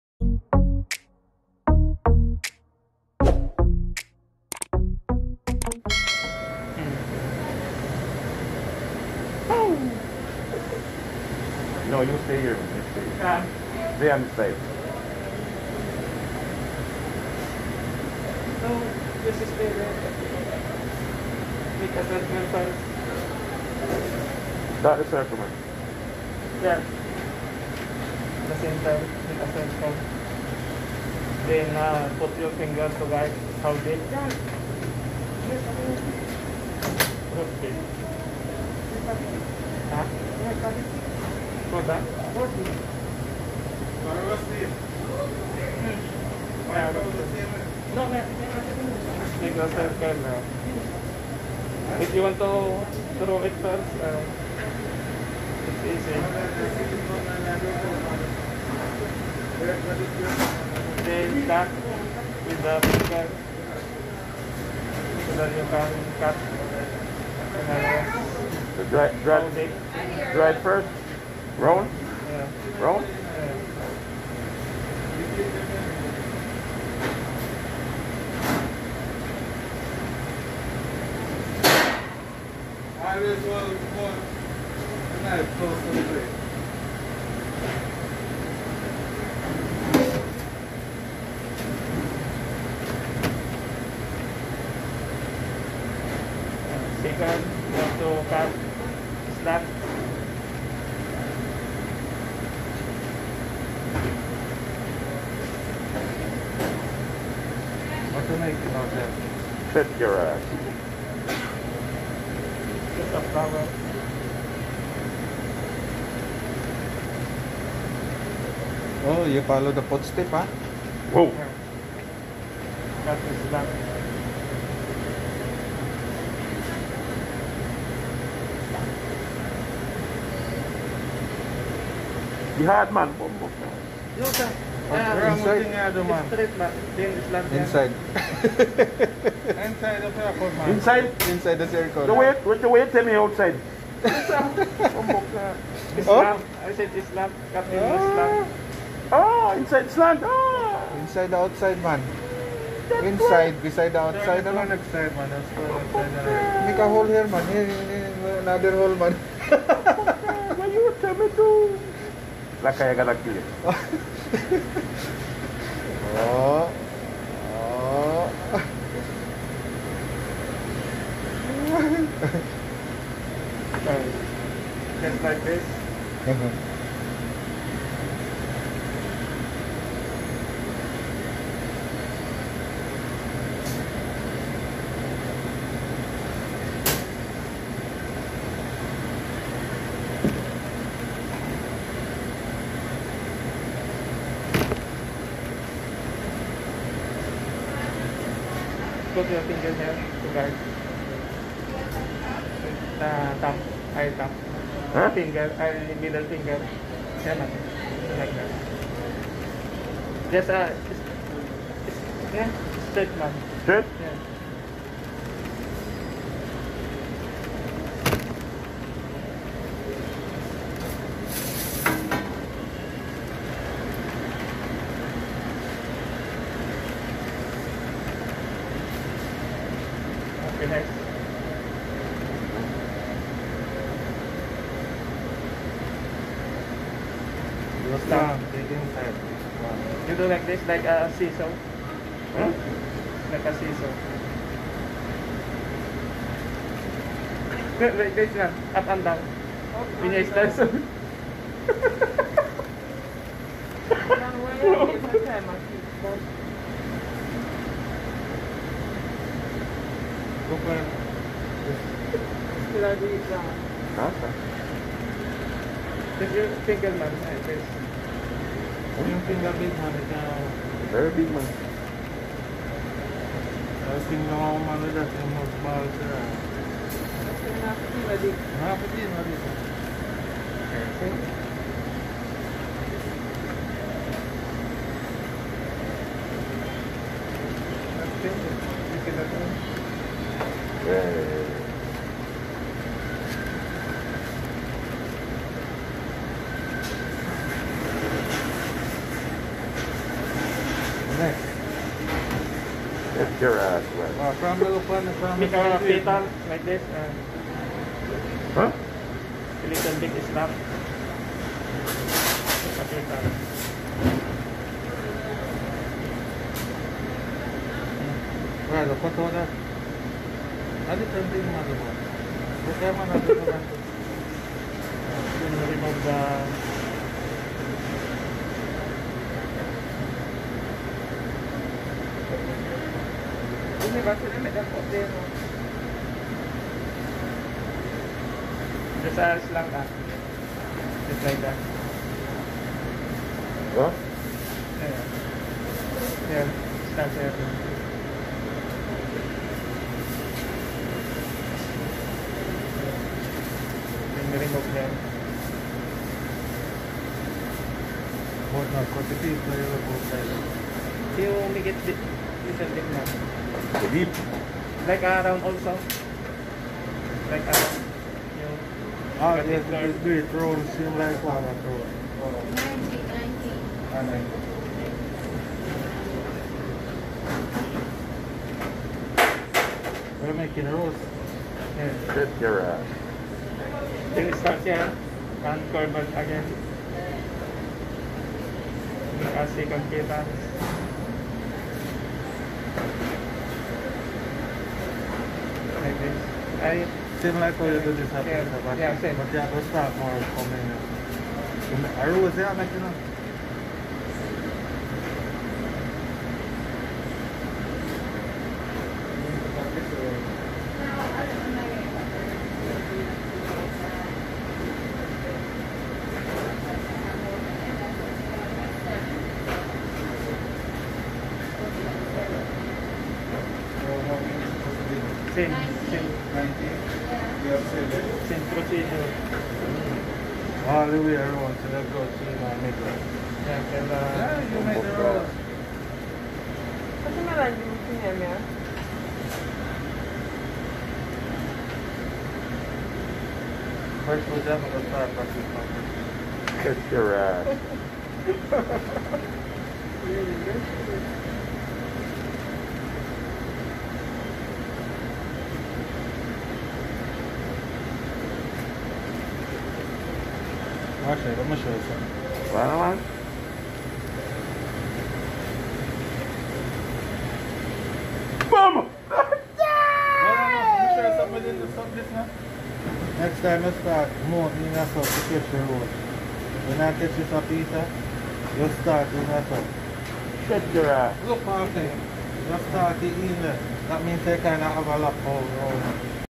Part 1: 🍉🍉🍉Making watermelon carving with Jamaican co-worker
I taught my co-worker how to carve simple watermelon rose flower. Doing this task you have need of patience and determination.